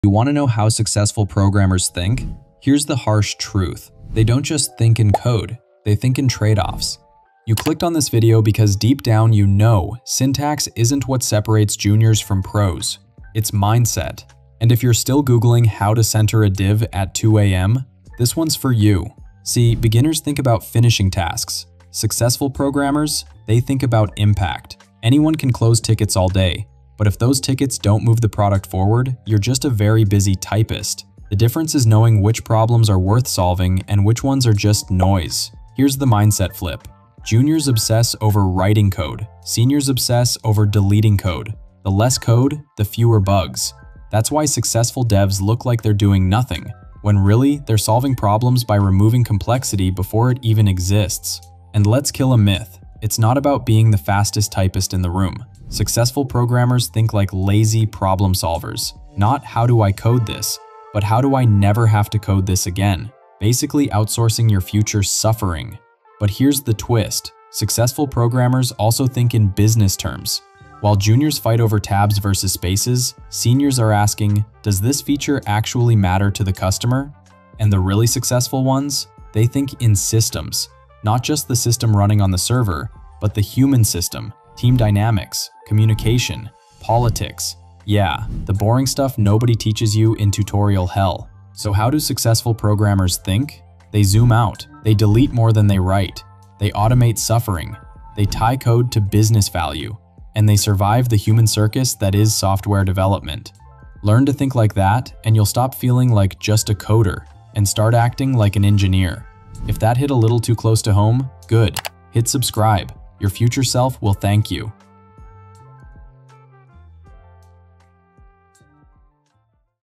How Successful Programmers Think
So you want to know how successful programmers think? Here’s the harsh truth… they don’t just think in code. They think in TRADE-OFFS. You clicked on this video because deep down you know… syntax isn’t what separates juniors from pros. It’s MINDSET. And if you’re still Googling “how to center a div” at 2 AM… this one’s for you.

Inspiration:
Shade of Code, Fireship